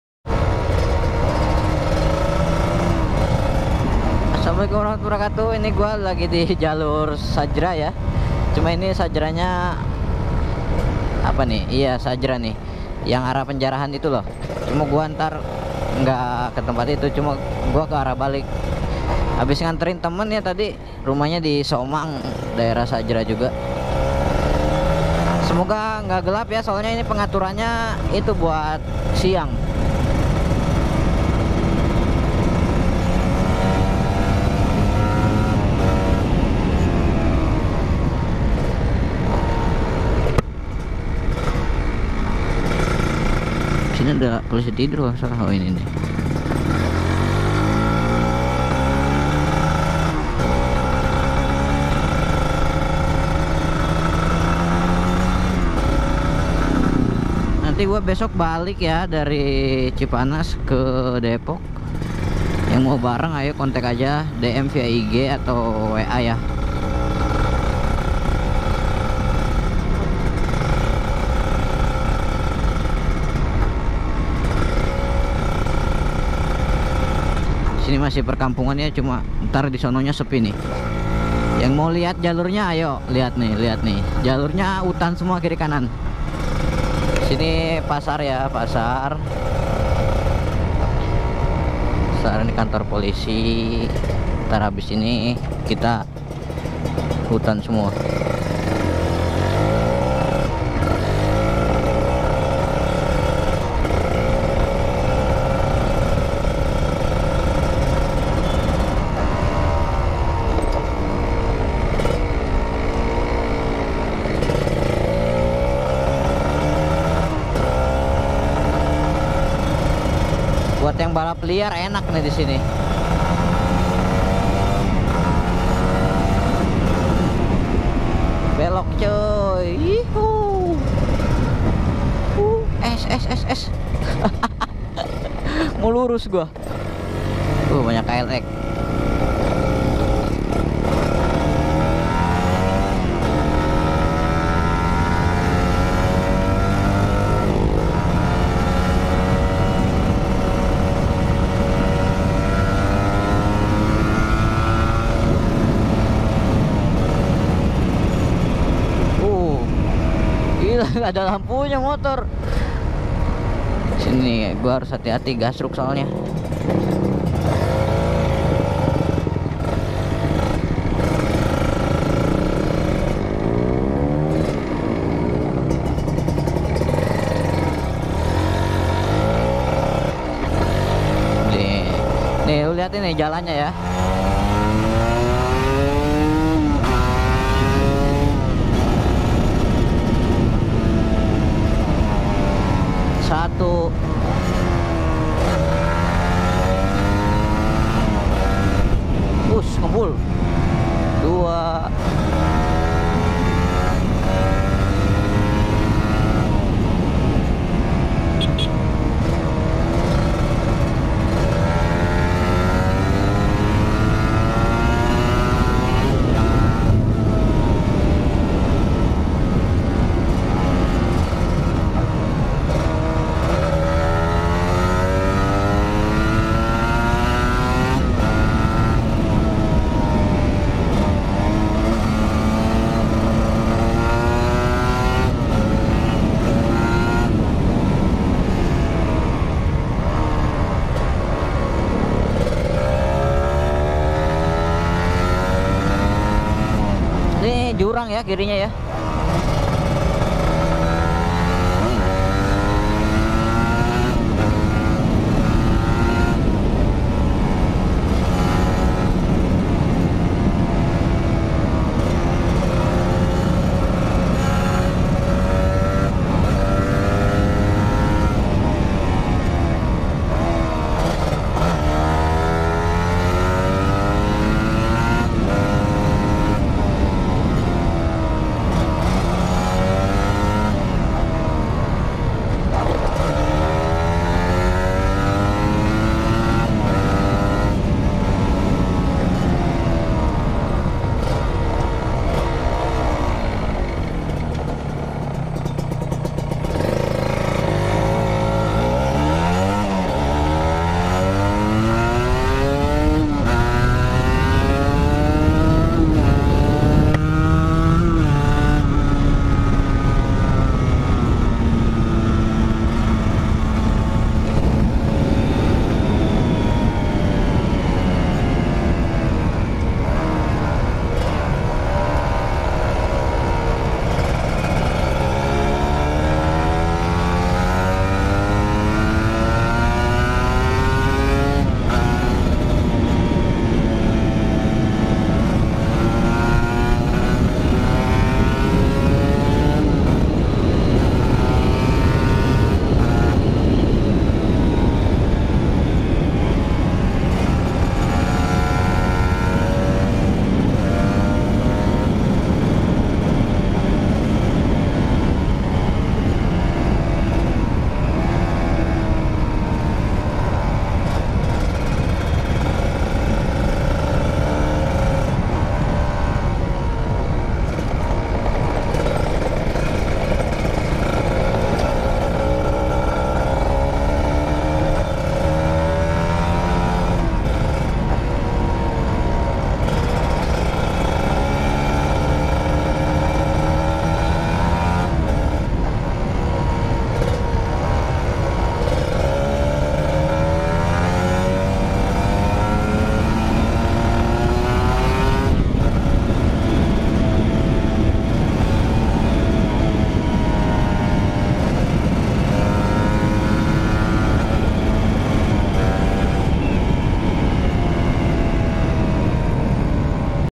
ANGKER||JALAN SAJIRA-MAKAM RIBEN
Kali ini gue nyobain jalur angker sajira makam riben, Hampir crash!!